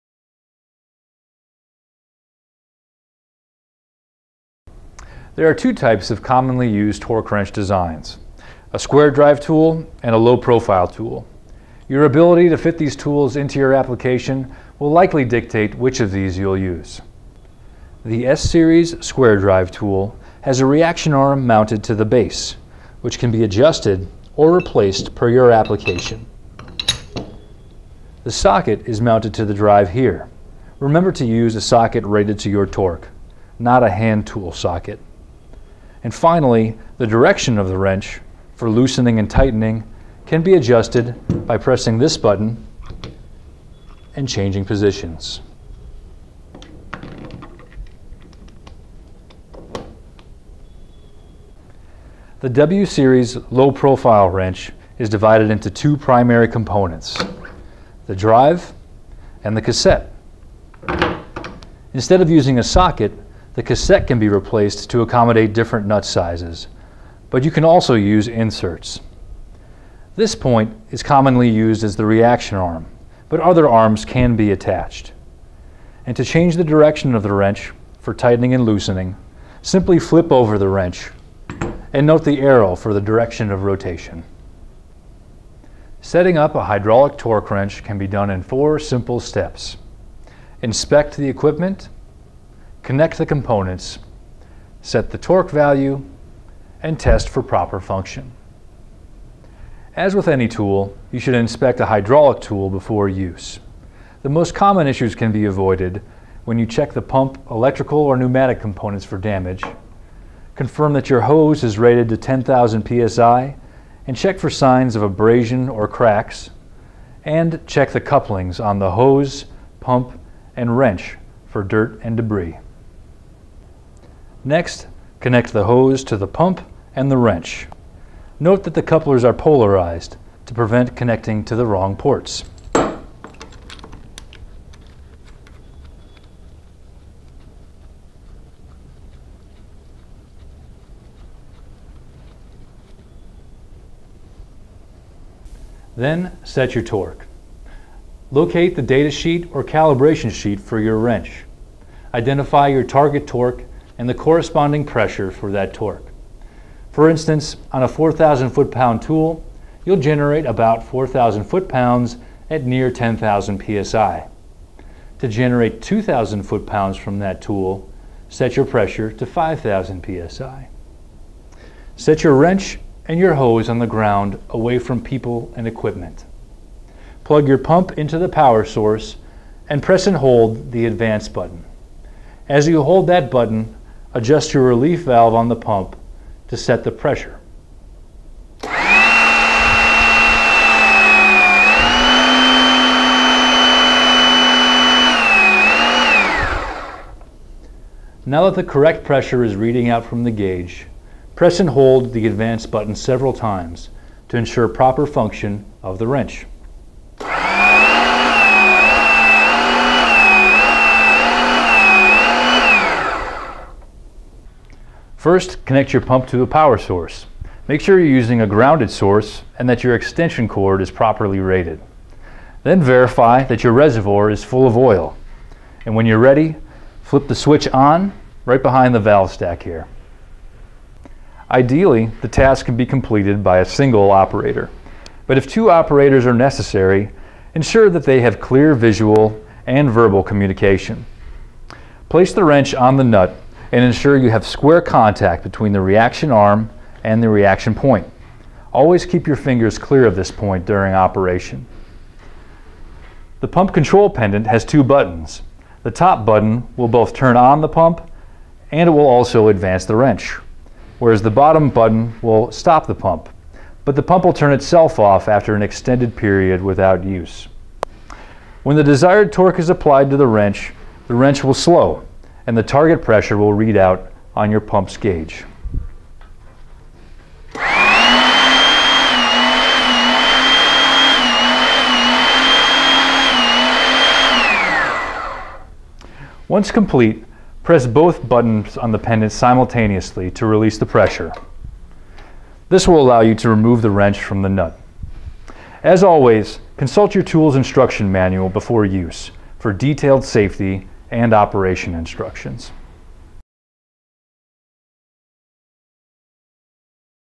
S & W Series Hydraulic Torque Wrenches Tool Demo | Enerpac
Simple overview demonstration of the  S & W Series Hydraulic Torque Wrenches